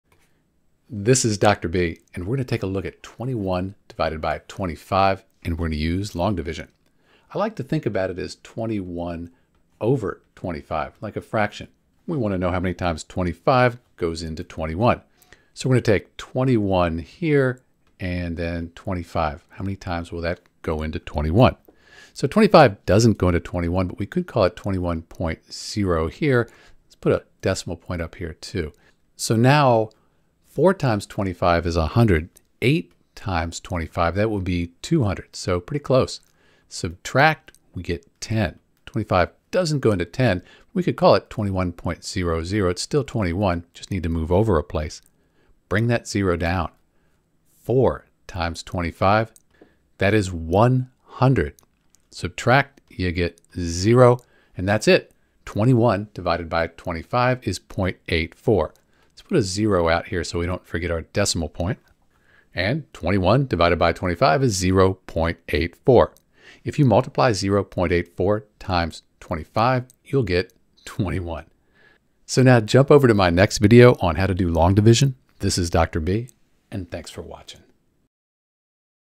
21 Divided by 25 (21 ÷ 25) Using Long Division – Step-by-Step Tutorial
In this video, we will divide 21 by 25 (21 ÷ 25) using long division. Let's walk through the steps to find the correct quotient.

For 21 Divided by 25:
📍 Dividend: 21
This is the number you want to divide.

📍 Divisor: 25
This is the number you are dividing by.

📍 Quotient: 0.84
This is the result of the division.

🛠️ Process:
1️⃣ Set Up the Division
Write 21 as the dividend inside the long division bracket and 25 as the divisor outside.

2️⃣ Determine How Many Times 25 Fits into 21
Since 25 is larger than 21, it does not fit. Write 0 in the quotient and place a decimal point.

3️⃣ Add a Zero and Continue
Add a zero to make it 210. Now determine how many times 25 fits into 210.

4️⃣ Divide
25 fits into 210 eight times because 25 × 8 = 200. Write 8 in the quotient.

5️⃣ Subtract and Continue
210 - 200 = 10. Bring down another zero to make 100.

6️⃣ Divide Again
25 fits into 100 exactly four times because 25 × 4 = 100. Write 4 in the quotient.

7️⃣ Subtract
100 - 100 = 0, so we’re done.

🧮 Final Quotient (Your Answer):
0.84

🔍 Checking Your Work:
Multiply 0.84 × 25 = 21 to verify the answer.